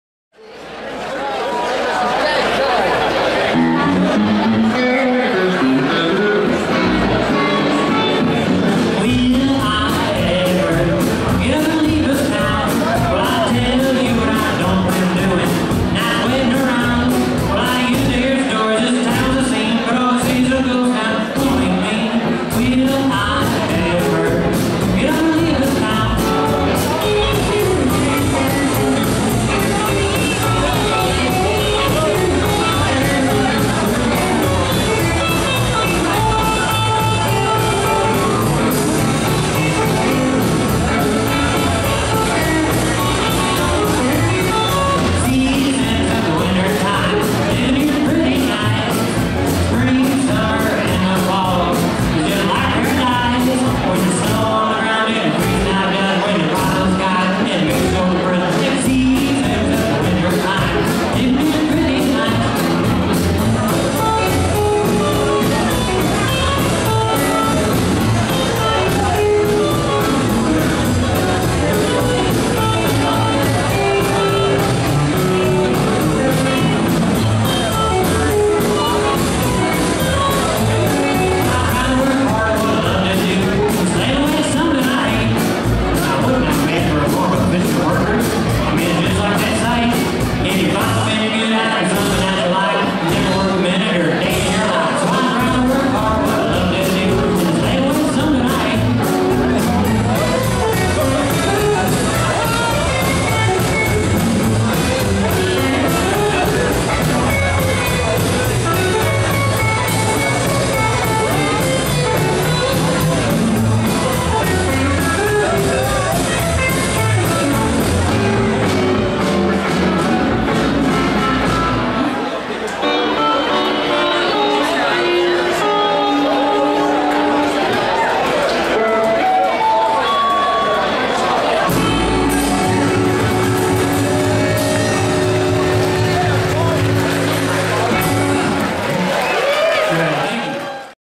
Kyle Weldridge live at the Nashville Boogie
Nashville Palace Kyle Eldridge live Dakota Collins on Bass